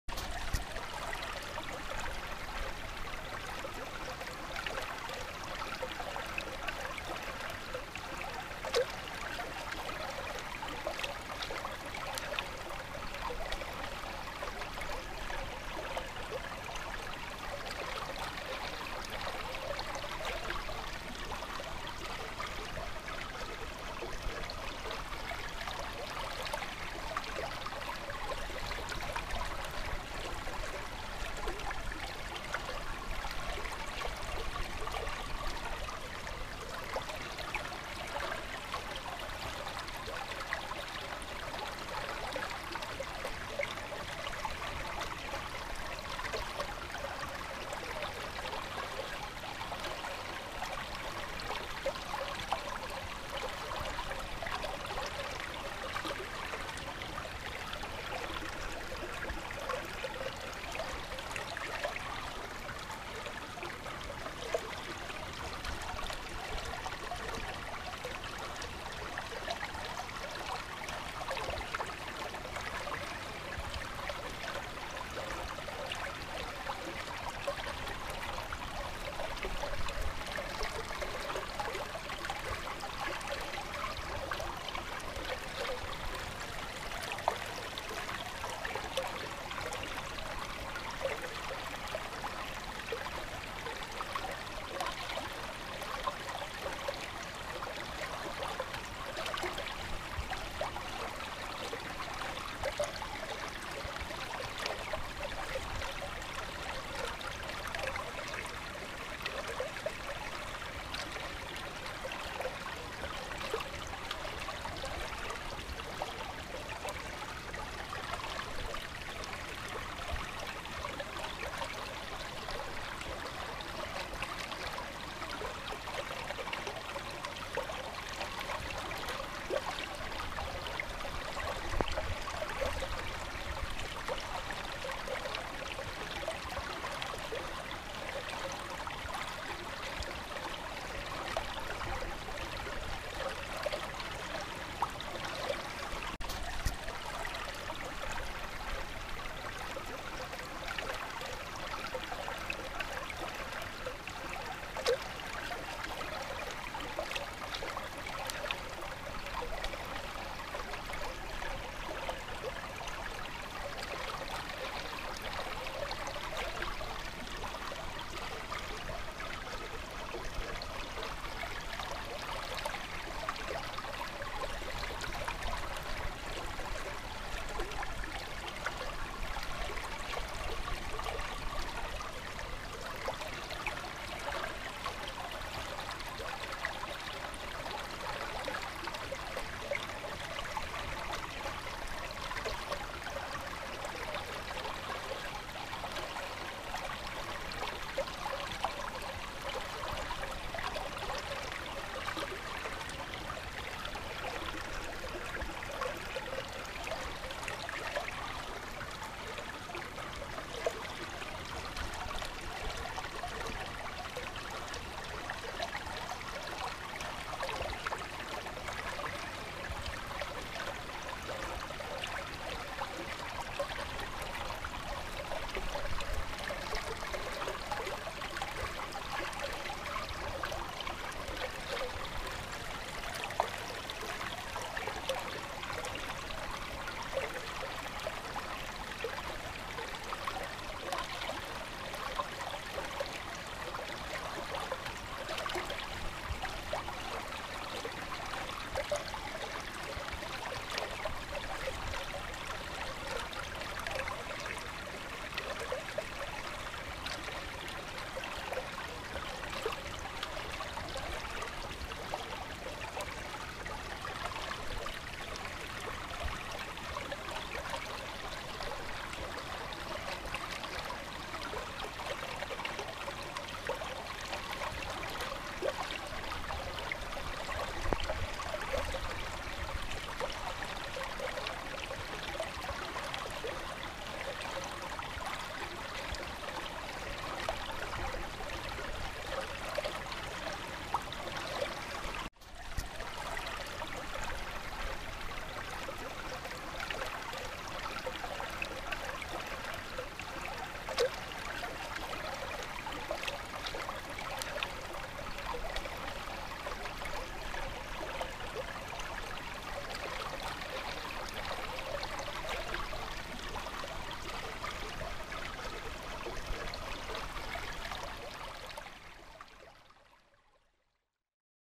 Beaver Dam Construction
This video was taken over several days and shows the Beaver's daily progress. Our beavers are nocturnal so all work they do is at night.